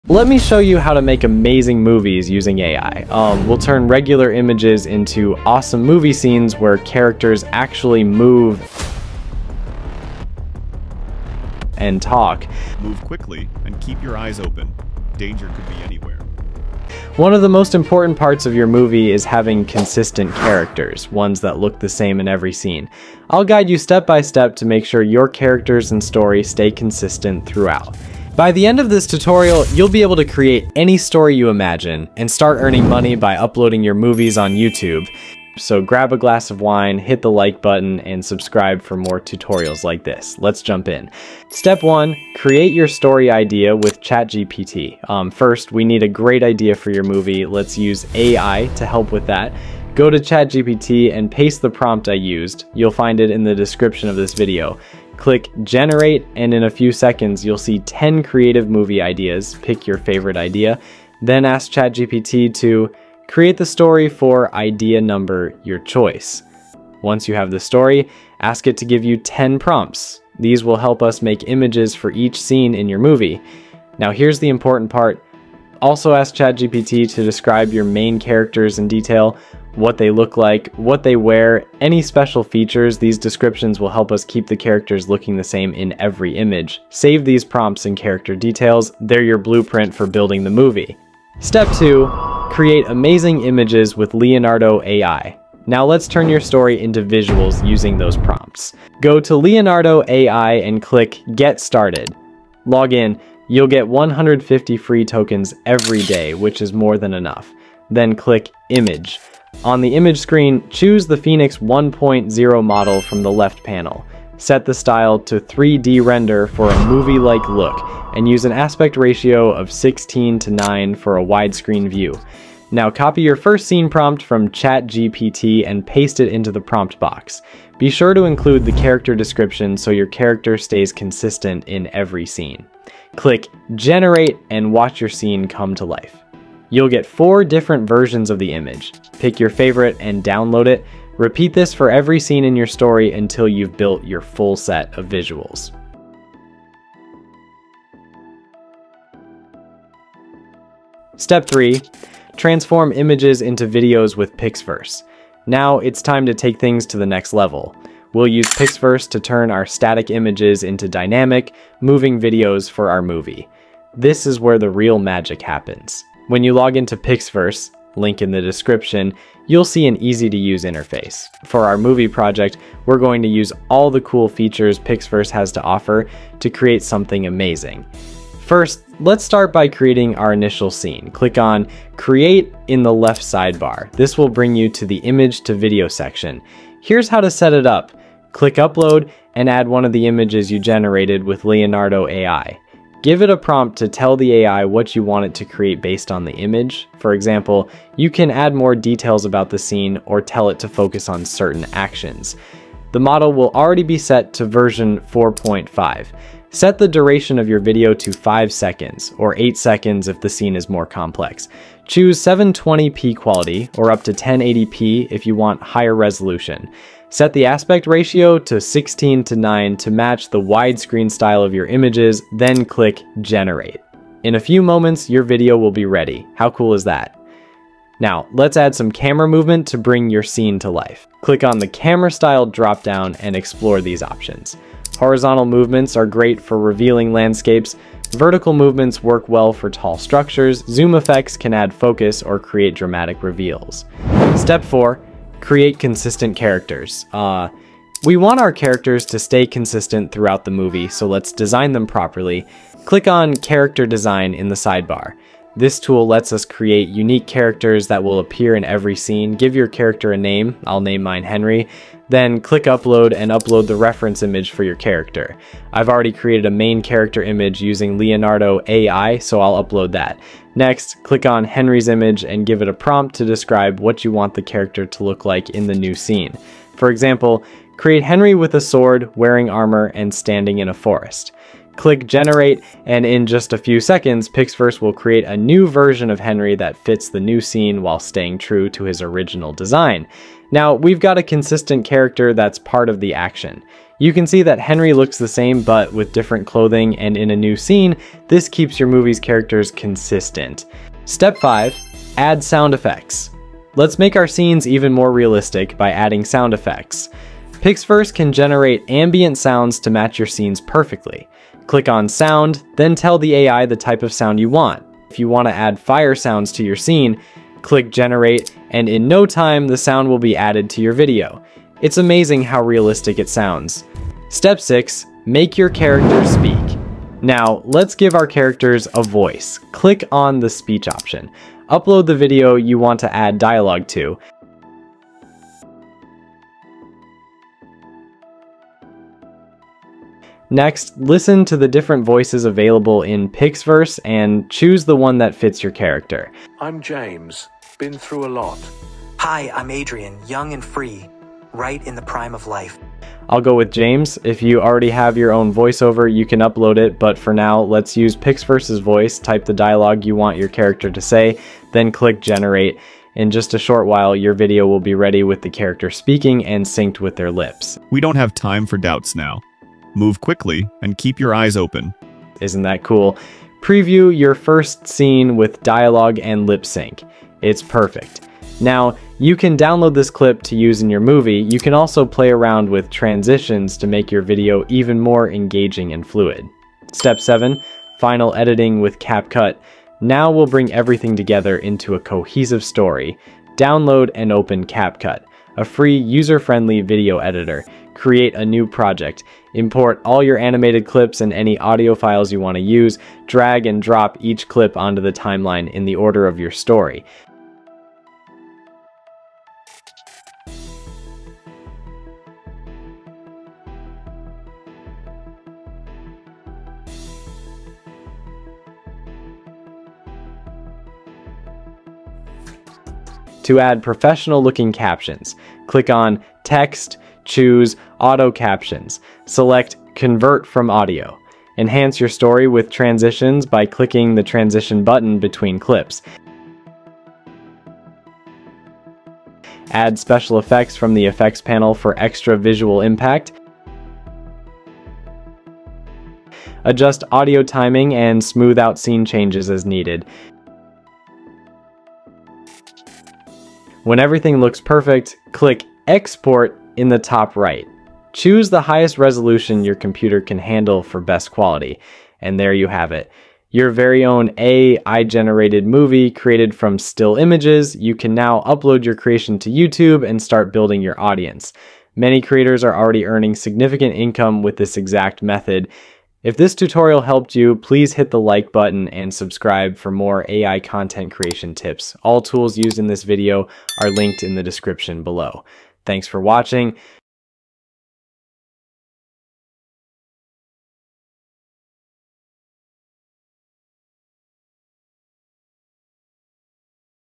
How to Create AI Films with CONSISTENT Characters | Step by Step
How to Create a Full AI-Generated Film from Scratch | Step-by-Step Tutorial for Beginners

🔥 Free Tools to Boost Your Content!

00:00 Intro
00:42 Step one
01:32 Step two
02:32 Step three
04:04 Step four
05:02 Step five
05:25 Step six
06:28 Step seven